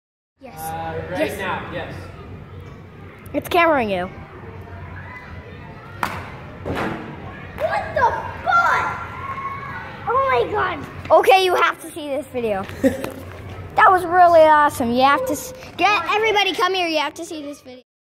Kid Stunned By Incredible Golf Trick Shot
ID: 1497726

An eight-year-old dumbstruck golf student was so surprised by his tutor’s great trick shot that he shouted ‘what the butt’ at the top of his lungs.

Golf professional and part-time kids coach, Nicolas Trice, had promised his class of young golf students that they would have an ice cream party if he could hit the golf ball into the basketball hoop at the apposing end of the sports hall.

The young boy who attended the after-school class in Austin, Texas, was so shocked by the great shot that he lost control of his reaction and slipped out the frowned upon saying, on April 11.

Although the entire class soon realised something naughty had been said, they were distracted by the fact the swear had been caught on camera and immediately wanted to watch it back.